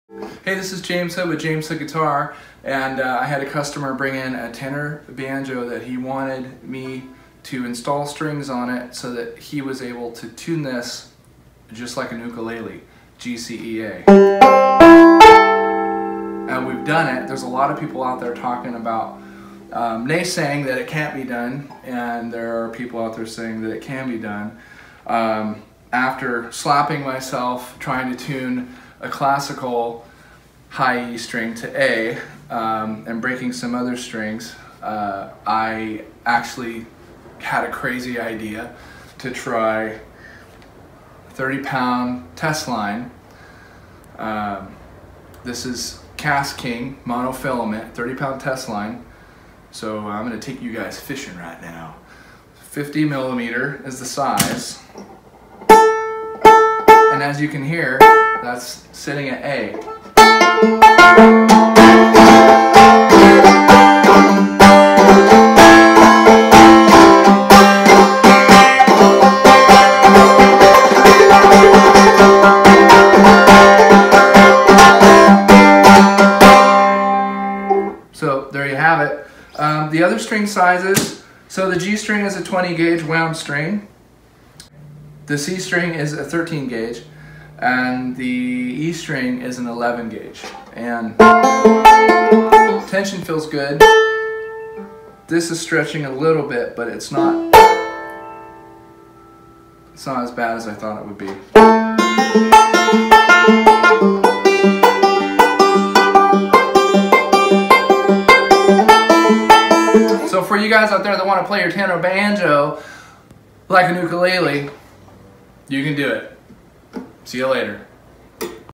Tune tenor banjo like ukulele
Tune your tenor-banjo like an ukulele with this little trick I learned with 30lb test line.